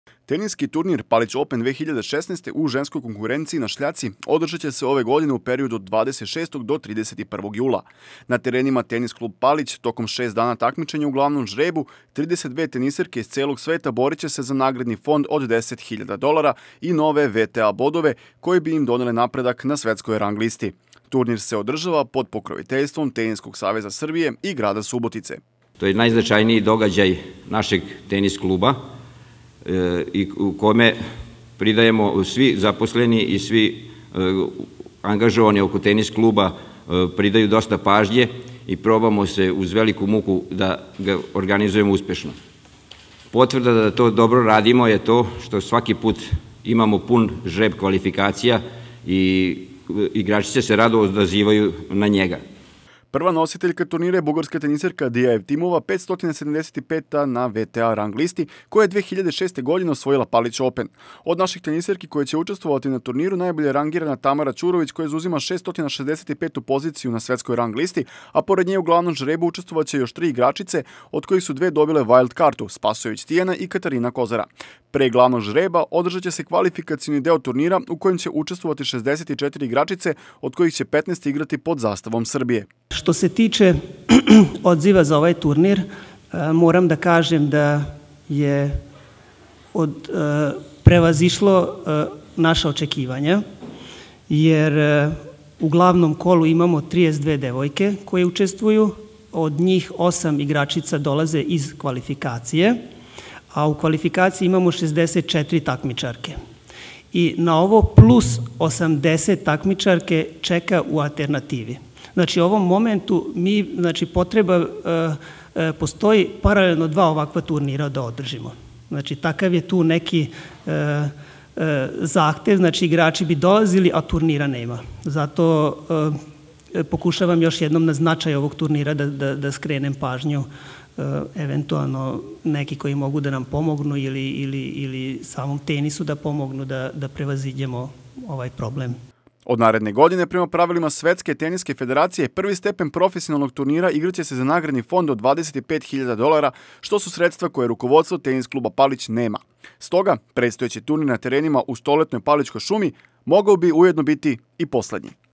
Palić Open od 26. do 31. jula
Teniski turnir Palić Open 2016. u ženskoj konkurenciji na šljaci, održaće se ove godine u periodu od 26. do 31. jula. Na terenima TK Palić tokom šest dana takmičenja u glavnom žrebu, 32 teniserke iz celog sveta boriće se za nagradni fond od 10.000 dolara i nove WTA bodove koji bi im doneli napredak na svetskoj rang listi. Turnir se održava pod pokroviteljstvom Teniskog saveza Srbije i Grada Subotice.

Prva nositeljka turnira je bugarska teniserka Dia Evtimova, 575. na WTA rang listi, koja je 2006. osvojila Palić Open.  Od naših teniserki, koje će učestvovati na turniru, najbolje je rangirana Tamara Ćurović, koja zauzima 665. poziciju na svetskoj rang listi, a pored nje u glavnom žrebu učestvovaće još tri igračice, od kojih su dve dobile vajld kartu, Tijana Spasojević i Katarina Kozara. Pre glavnog žerba, održaće se kvalifikacioni deo turnira u kom će učestvovati 64 igračice od kojih će 15 igrati pod zastavom Srbije.

Od naredne godine, prema pravilima Svetske teniske federacije, prvi stepen profesionalnog turnira igraće se za nagradni fond od 25.000 dolara, što su sredstva koja rukovodstvo TK Palić nema. Stoga, predstojeći turnir na terenima u stoletnoj Palićkoj šumi, mogao bi ujedno biti i poslednji.